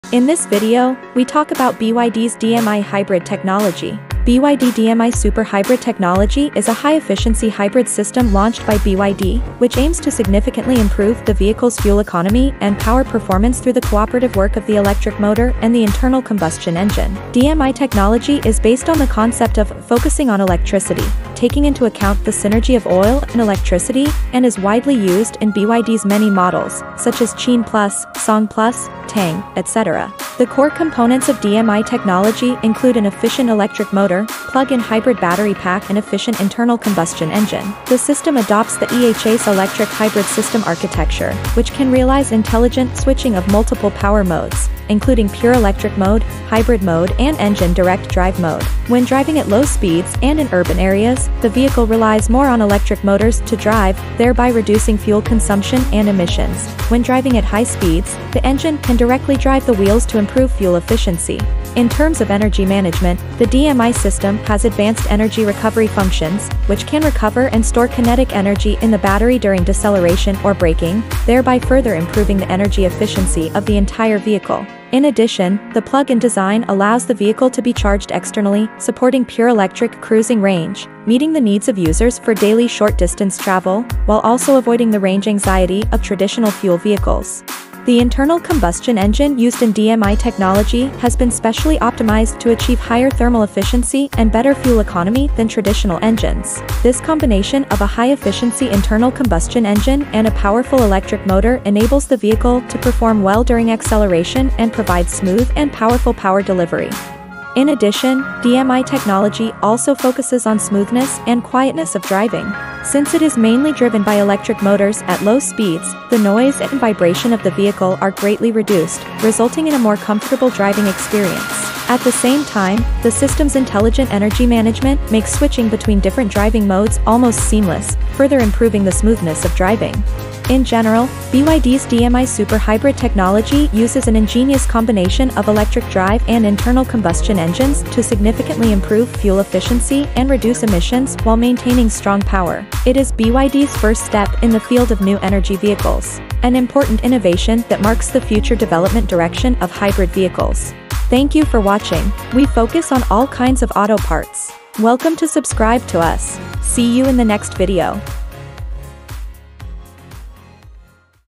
BYD DM-i Super Hybrid Technology#auto
Asiautos Auto Parts present
BYD DM-i super hybrid technology is a high-efficiency hybrid system launched by BYD, which aims to significantly improve the vehicle's fuel economy and power performance through the cooperative work of the electric motor and the internal combustion engine. DM-i technology is based on the concept of "focusing on electricity, taking into account the synergy of oil and electricity" and is widely used in BYD's many models, such as Qin PLUS, Song PLUS, Tang, etc.
The core components of DM-i technology include an efficient electric motor, plug-in hybrid battery pack and efficient internal combustion engine. The system adopts the EHS electric hybrid system architecture, which can realize intelligent switching of multiple power modes, including pure electric mode, hybrid mode and engine direct drive mode. When driving at low speeds and in urban areas, the vehicle relies more on electric motors to drive, thereby reducing fuel consumption and emissions; when driving at high speeds, the engine can directly drive the wheels to improve fuel efficiency.
In terms of energy management, the DM-i system has advanced energy recovery functions, which can recover and store kinetic energy in the battery during deceleration or braking, thereby further improving the energy efficiency of the entire vehicle. In addition, the plug-in design allows the vehicle to be charged externally, supporting pure electric cruising range, meeting the needs of users for daily short-distance travel, while also avoiding the range anxiety of traditional fuel vehicles.
The internal combustion engine used in DM-i technology has been specially optimized to achieve higher thermal efficiency and better fuel economy than traditional engines. This combination of a high-efficiency internal combustion engine and a powerful electric motor enables the vehicle to perform well during acceleration and provide smooth and powerful power delivery.
In addition, DM-i technology also focuses on smoothness and quietness of driving. Since it is mainly driven by electric motors at low speeds, the noise and vibration of the vehicle are greatly reduced, resulting in a more comfortable driving experience. At the same time, the system's intelligent energy management makes switching between different driving modes almost seamless, further improving the smoothness of driving.
In general, BYD's DM-i super hybrid technology uses an ingenious combination of electric drive and internal combustion engines to significantly improve fuel efficiency and reduce emissions while maintaining strong power. It is BYD's first step in the field of new energy vehicles. An important innovation that marks the future development direction of hybrid vehicles.
Asiautos.net
your reliable partner for Asia auto parts